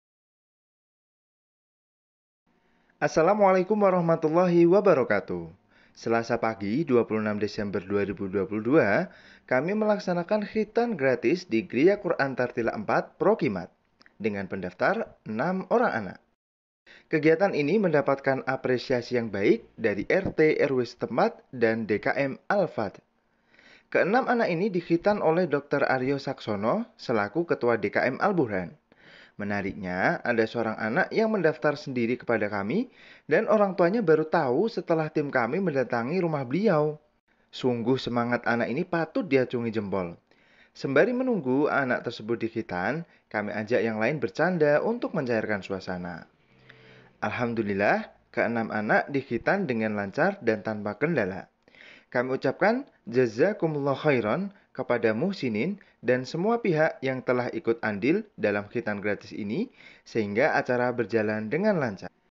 Khitan Masal Bersama Bass FM
Khitanan masal bersama Bass FM ini gratis, dan kami juga menyediakan layanan untuk kontrol. Khitan bersama Bass kami ini kami selenggarakan di GQT 4, Prokimad, Tengaran, Kab. Semarang pada hari Selasa, 27 Desember 2022

Baarakallahu fiikum..
Semoga bermanfaat.